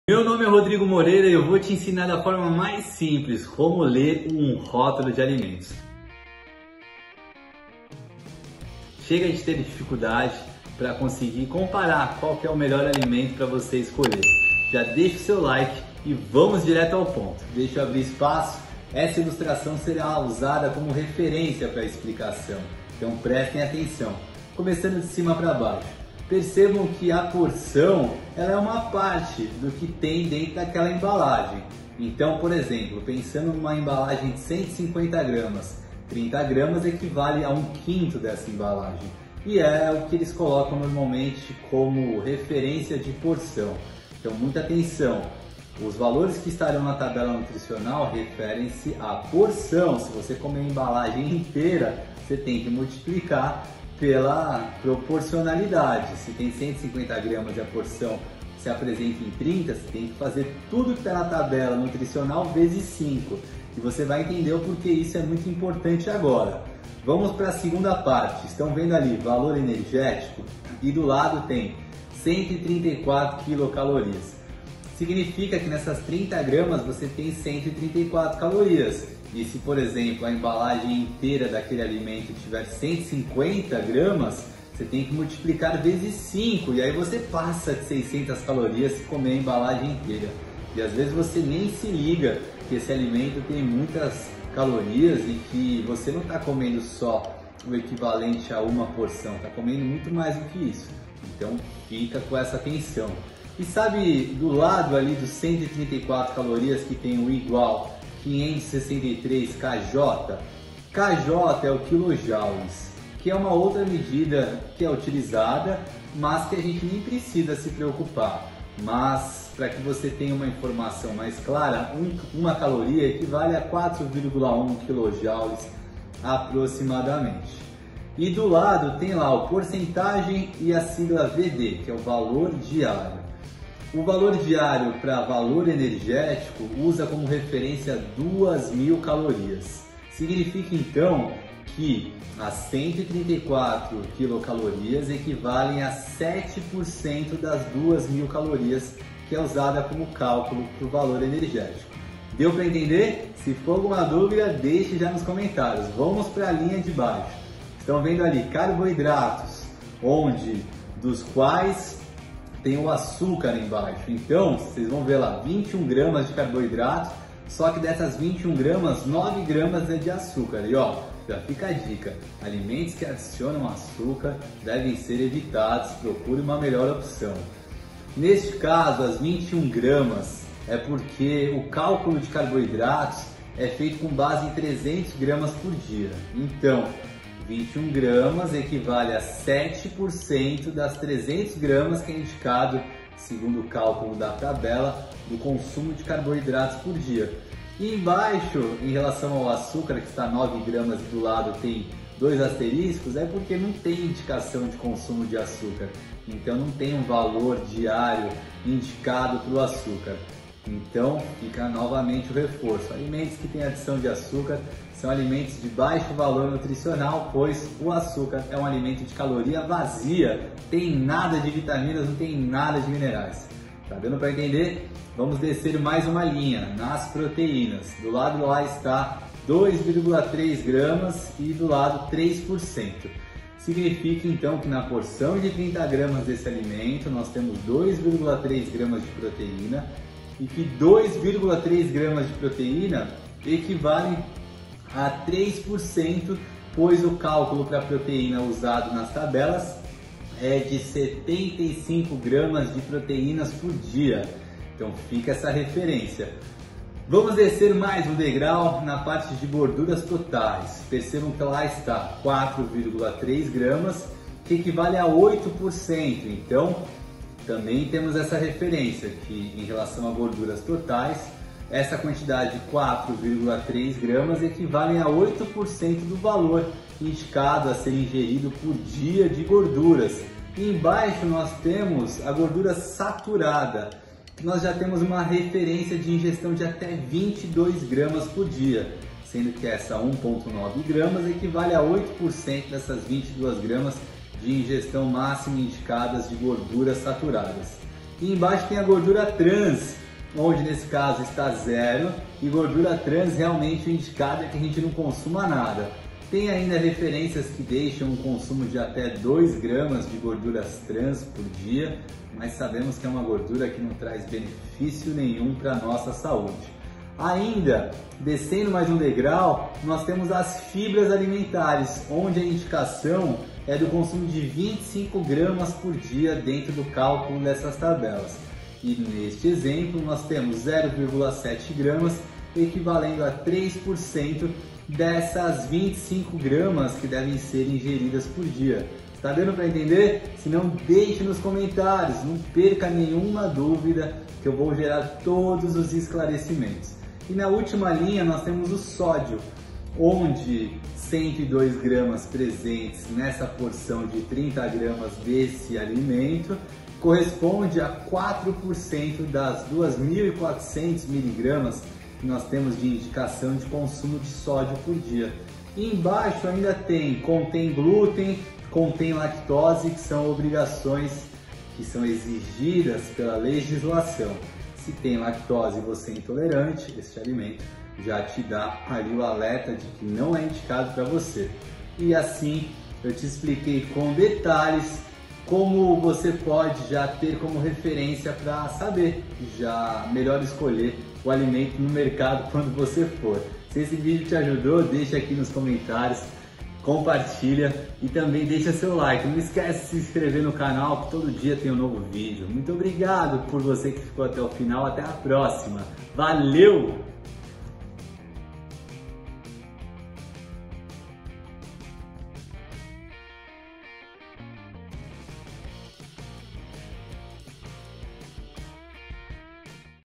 Como LER RÓTULO NUTRICIONAL? | Nutri Rodrigo Moreira
O rótulo dos alimentos é uma tabela com informações sobre as quantidades e qualidades de nutrientes como carboidratos, proteínas e vitaminas dos alimentos, além de explicar os ingredientes que são usados no preparo dos produtos.

Saber ler o rótulo dos alimentos facilita as decisões na hora de escolher um produto, porque permite comparar a quantidade e qualidade dos nutrientes para saber se um produto é saudável ou não, ajudando a manter um hábito alimentar saudável.

Entenda tudo nesse vídeo!

Acesse meus dois Instagram -
@nutrirodrigomoreira.chas e